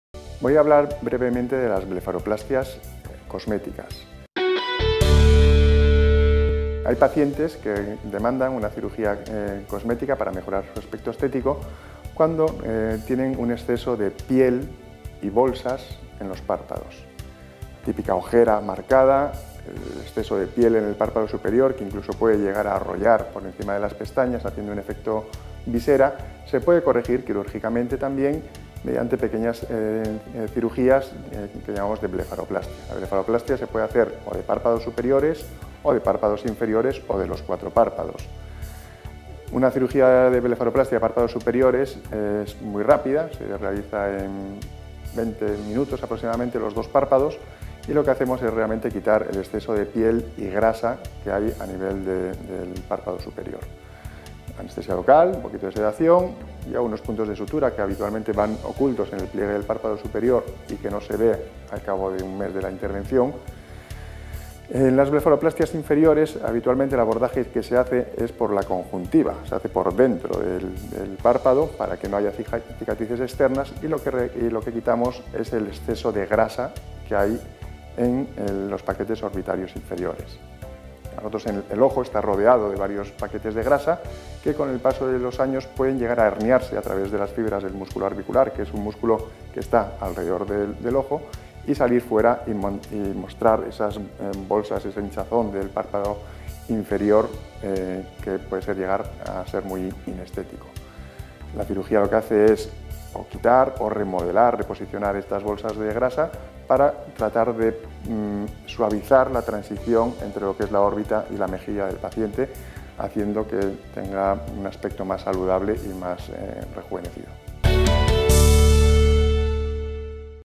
Dr. Javier Fernández-Vega: ¿Qué la blefaroplastia cosmética?
Hay pacientes que presentan excesos de piel y bolsas en los párpados. Se pueden corregir con cirugía de blefaropastia. El Dr. Javier Fernández-Vega explica en este vídeo en qué consiste.